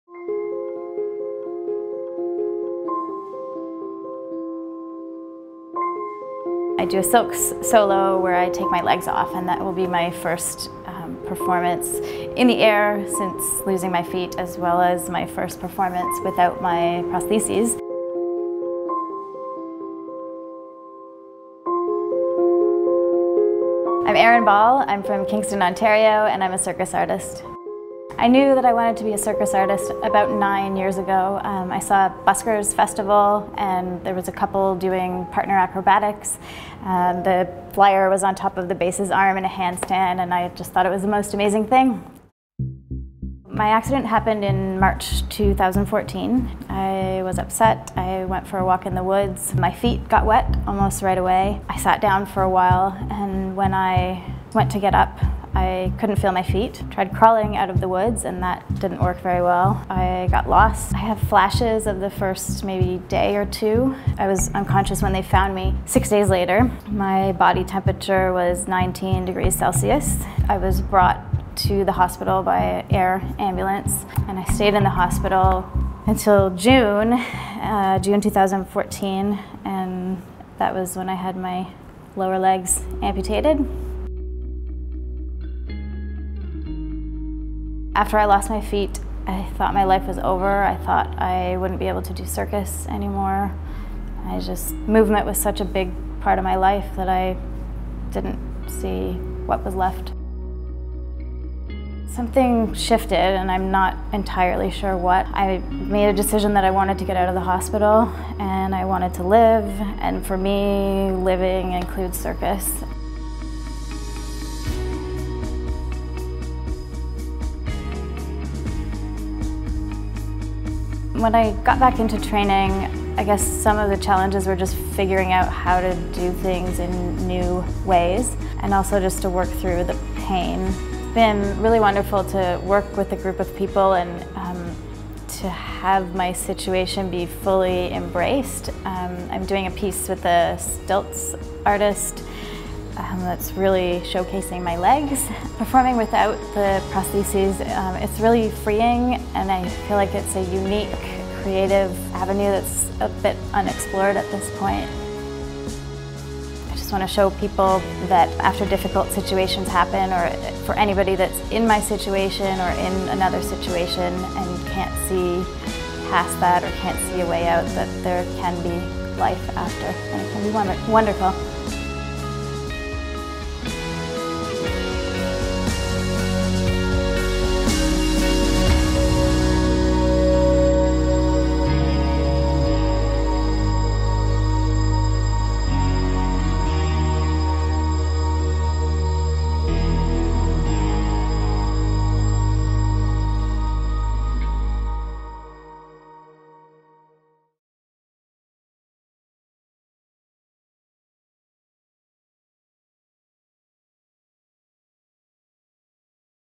Double Amputee Circus Artist Learns to Fly Again
Watch the extraordinary story of Erin Ball, a circus performer who got back in the ring after losing her lower legs.

Erin Ball had been performing as a circus artist for nine years before an accident in the woods led to the emergency amputation of her legs. After learning how to walk again she was determined to return to her passion for circus performing.In this segment, CBC Arts met Ball at a rehearsal for Circus Sessions 2016. She talks about her rehabilitation journey and about the freedom she's discovered in performing without her prostheses.